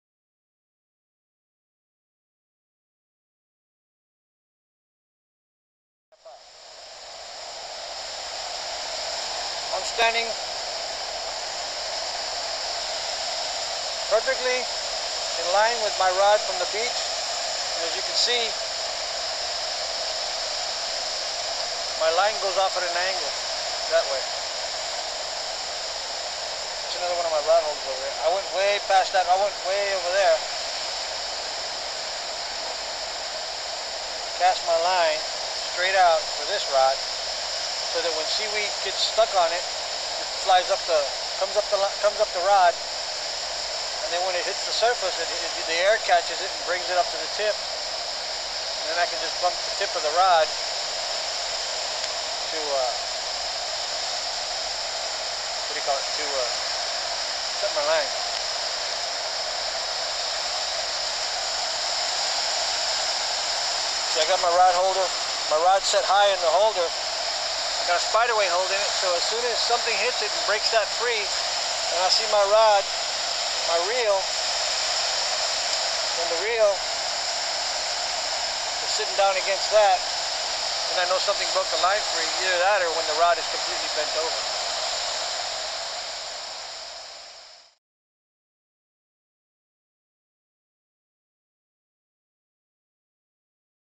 Dealing with seaweed while fishing
A lot of people refuse to fish when there is a lot of sea weed in the surf. Here is a solution to that problem... Simply enough, fish at an angle into the current as demonstrated by me MANUEL Z